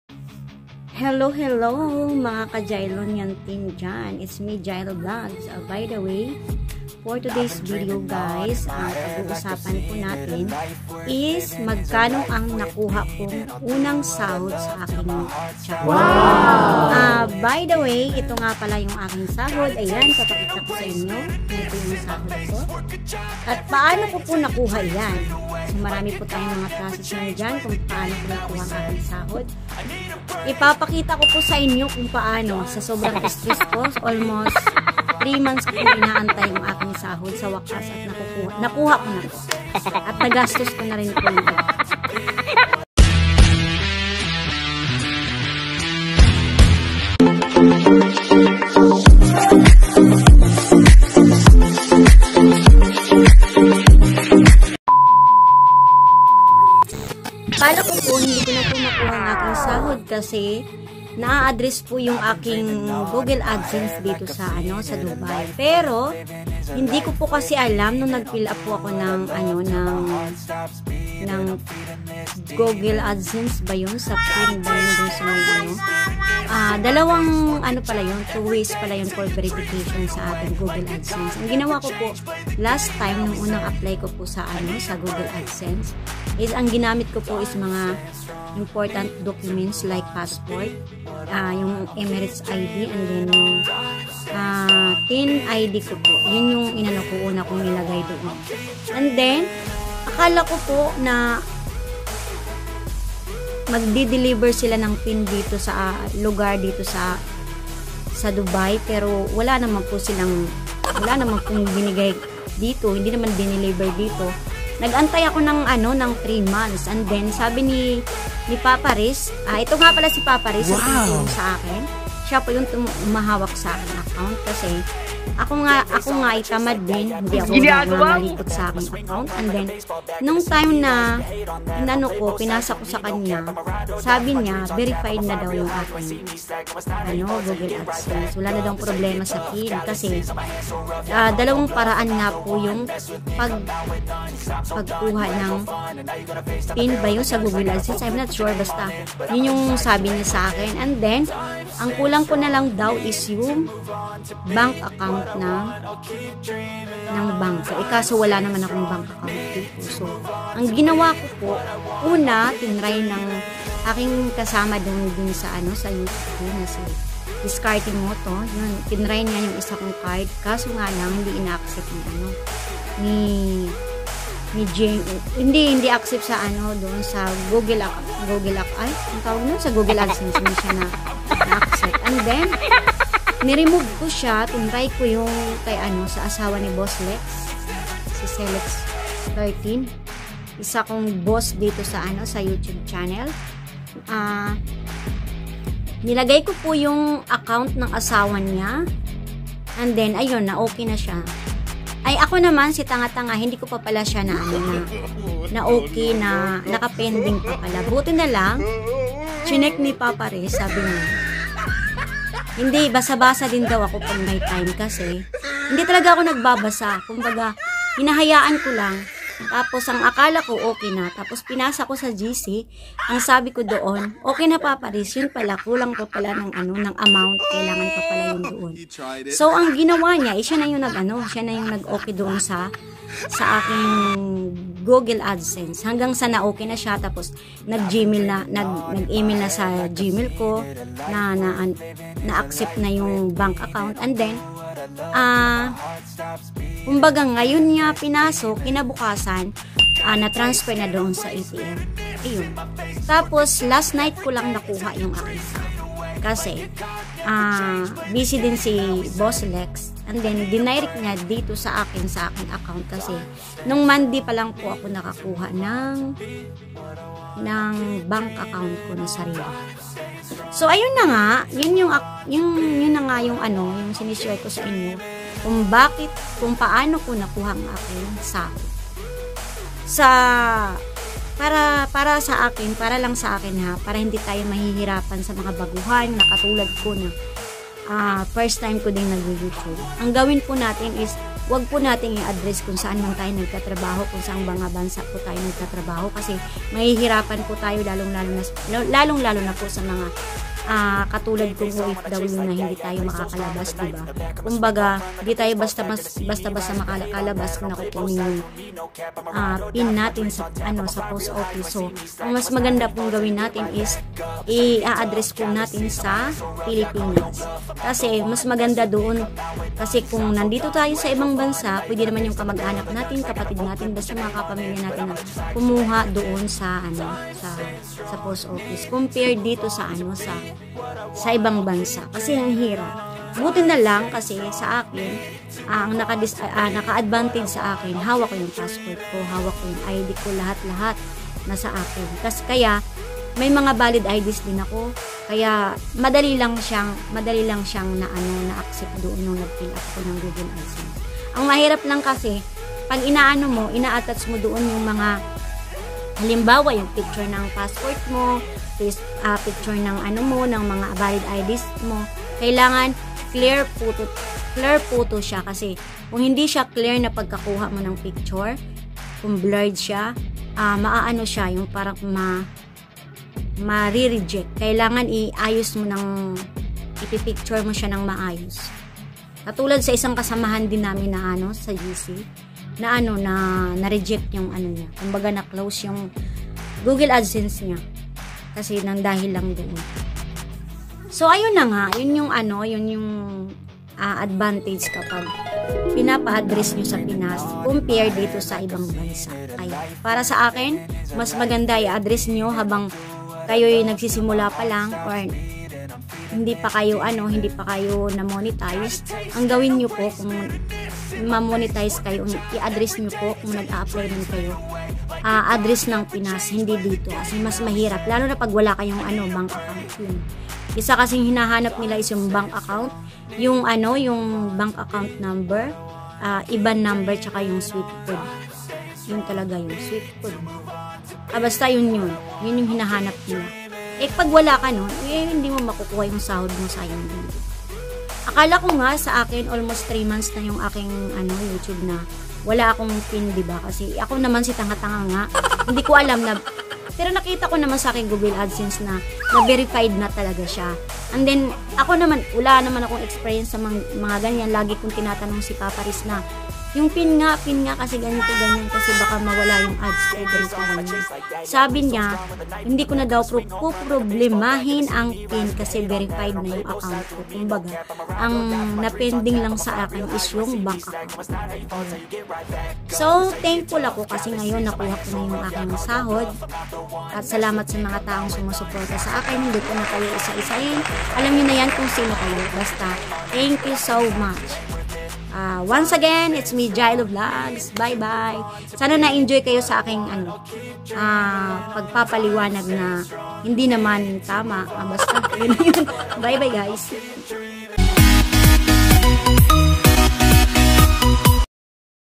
UNANG SAHOD KO SA YOUTUBE || MAY HARD WORK WAS PAID OFF!!! || JYLLO VLOGZ
youtube salary MY HARD WAS PAID OFF!! THANK YOU SA LAHAT NG MGA SUPPORTERS KO JAN NA LAGING ANJAN ITO NA NAKUHA KONA DN 🥰🥰🥰